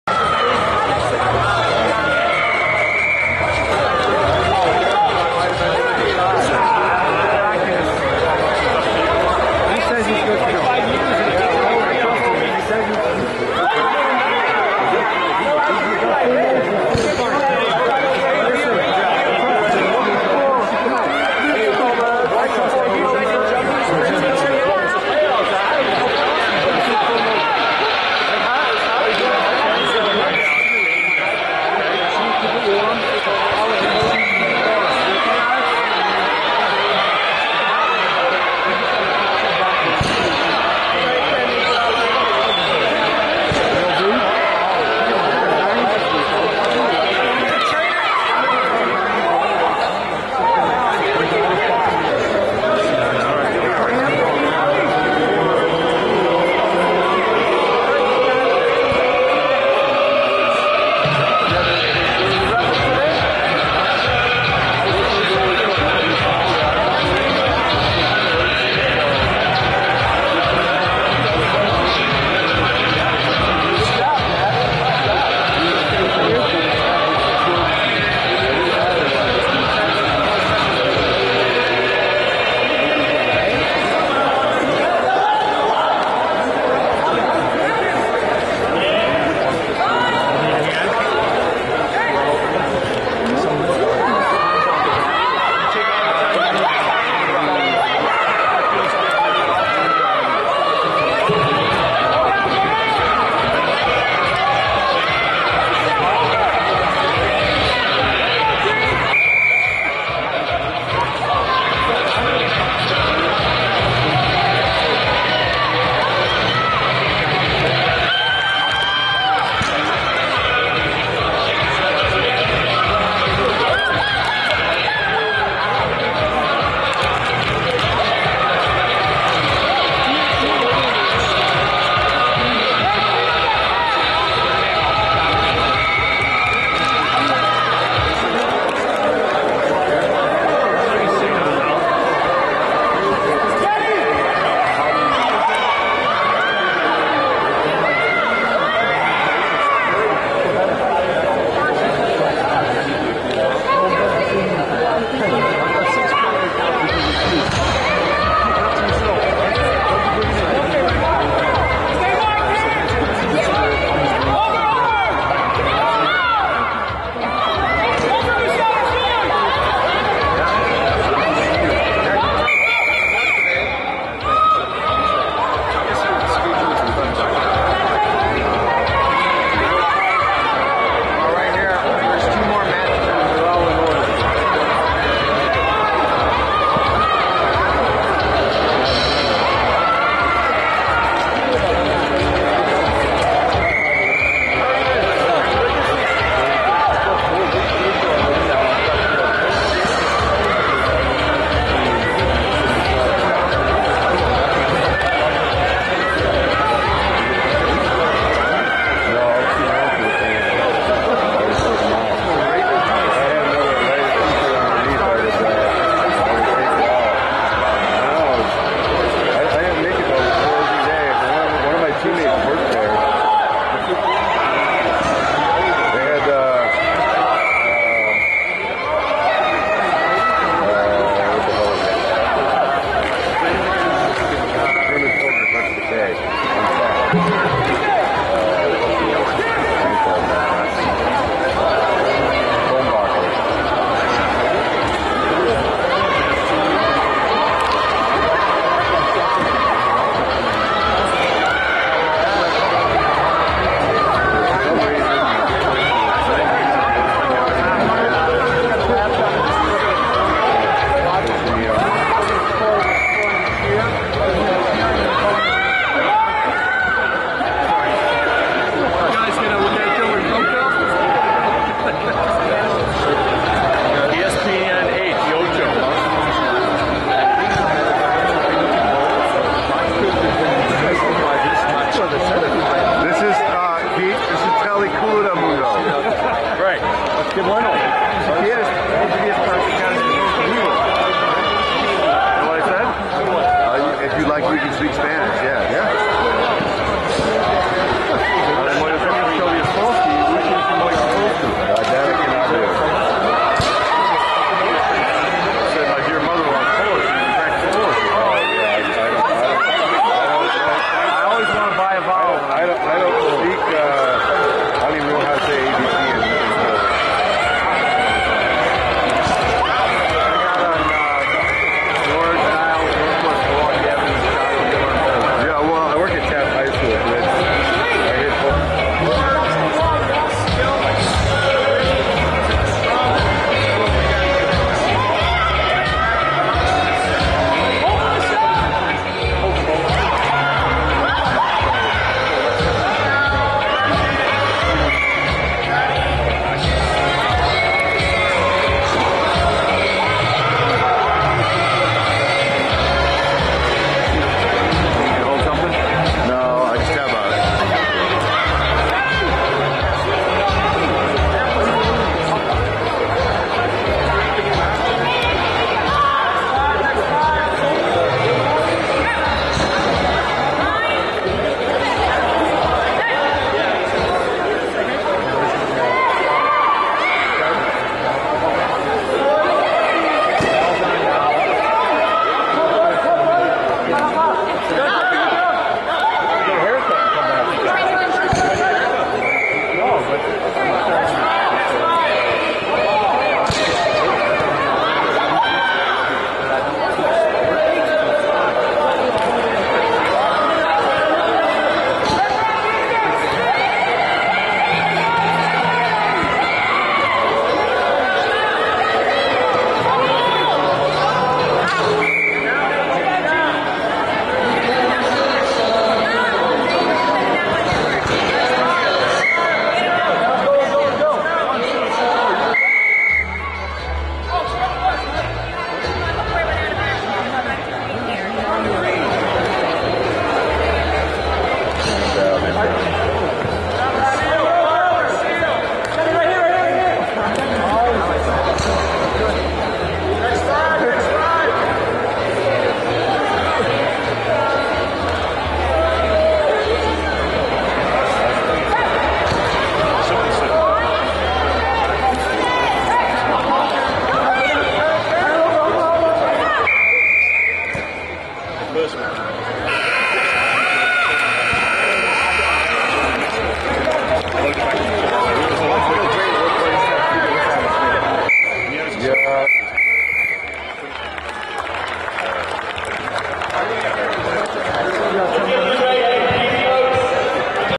HSG_CRC_v_Carmel
December 10th, 2022 Rugby X [5s] tournament Series Hosted by Silverback Nation Rugby